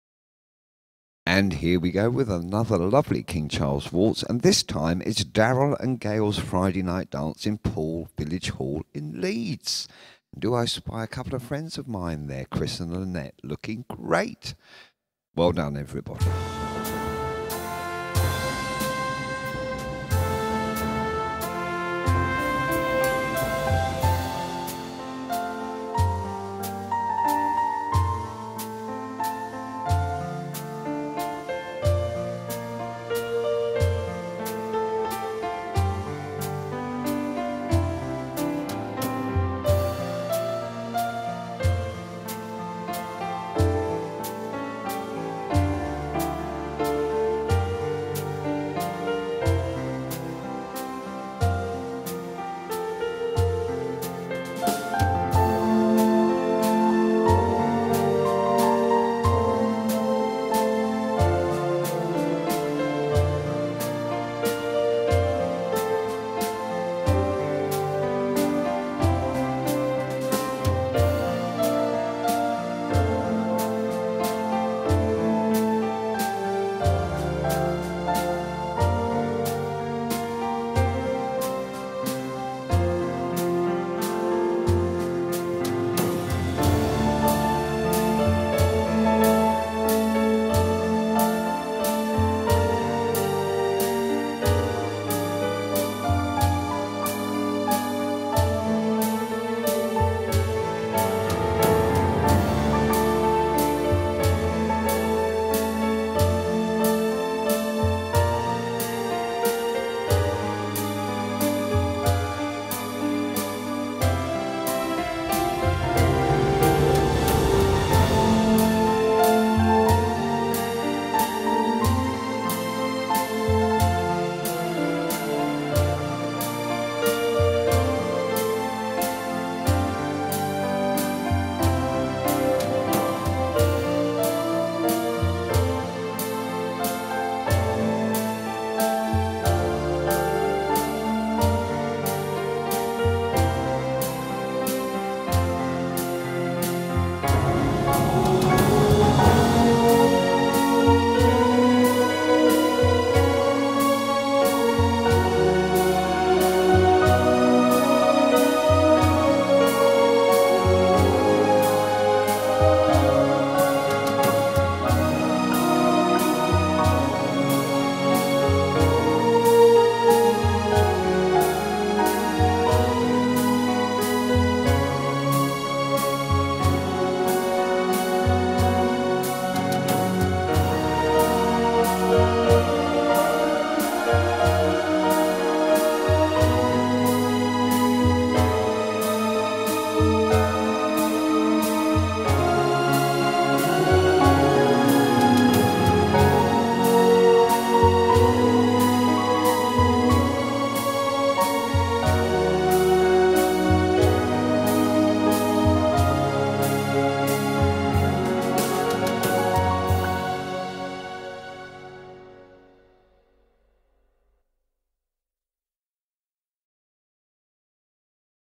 The King Charles Waltz " Poole Village Hall" Leeds. Darrell and Gayle
Here we go with another great King Charles Waltz. This time it's Darrell and Gayle's Friday night dance at Poole Village Hall in Leeds...well done everyone :)